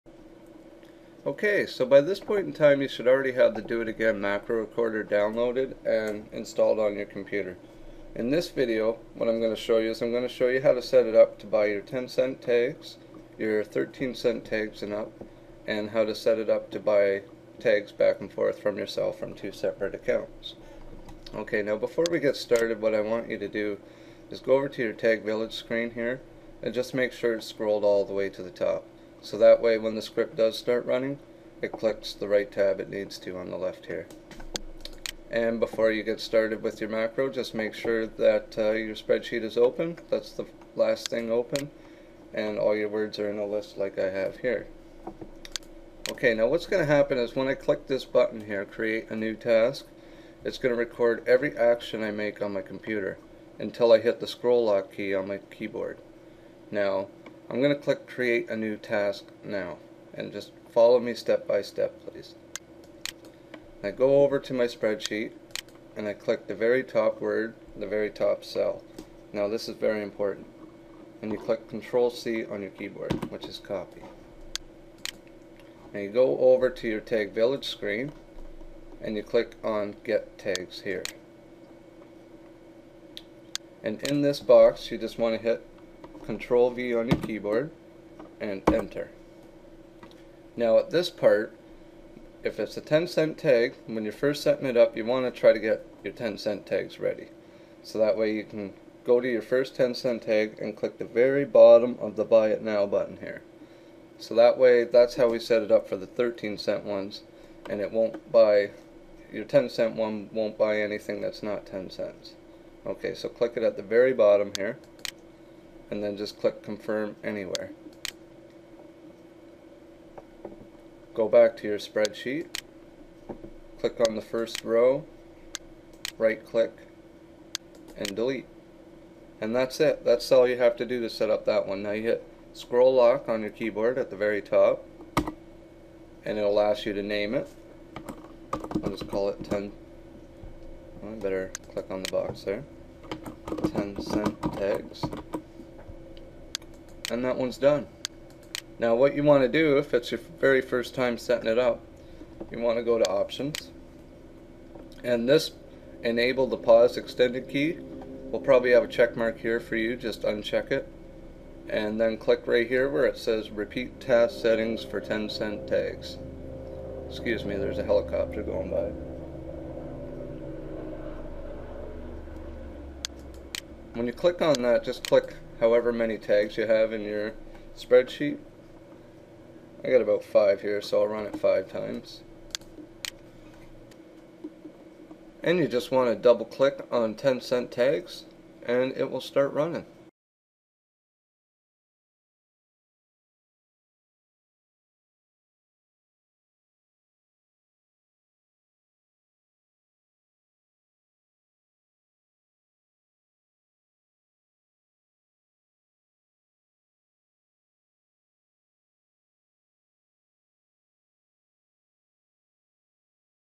Do It Again Macro Recorder For Tagvillage
In this video I am going to show you how to set up the do it again macro recorder for tagvillage to buy your 10c tags 13 c tags and up and to buy words back from yourself on a seperate accnt